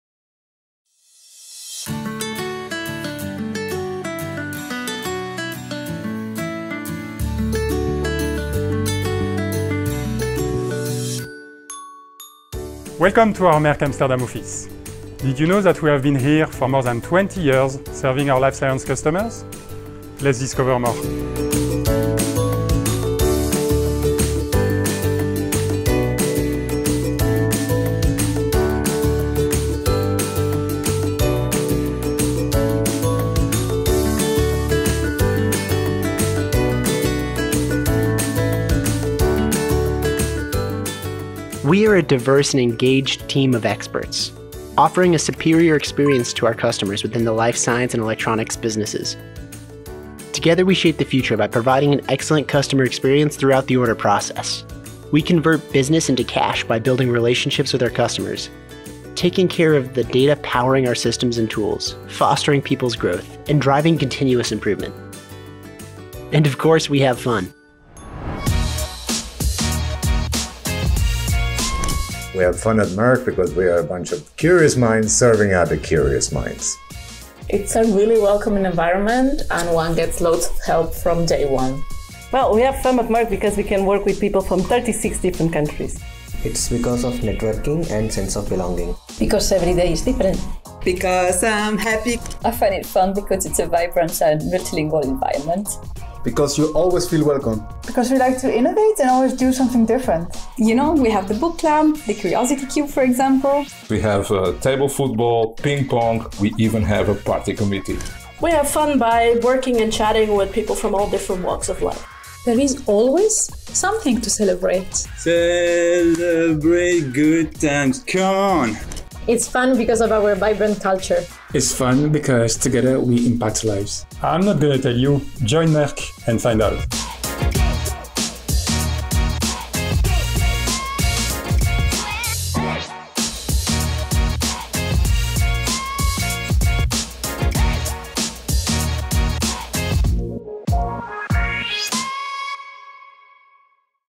Merck Chemicals BV Promotional Video - Our Life Science office in Amsterdam
Welcome to Merck Life Science office in Amsterdam!
Be curious and have a look at who we are, what we do and how we serve our Life Science while having fun at work